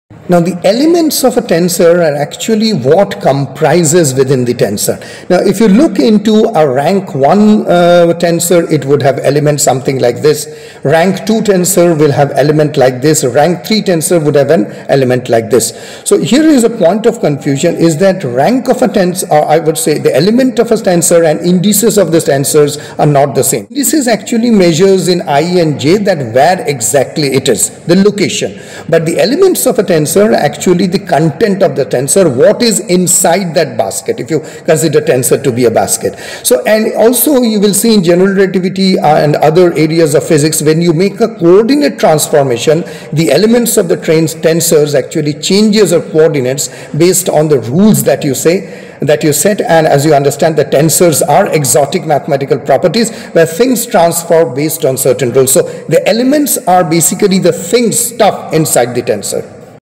What Are Elements Of A Tensor | Tensor Calculus
What are elements of a tensor. What are indices of a tensor. In this short video, you will learn, what are those and how it is useful in tensor analysis.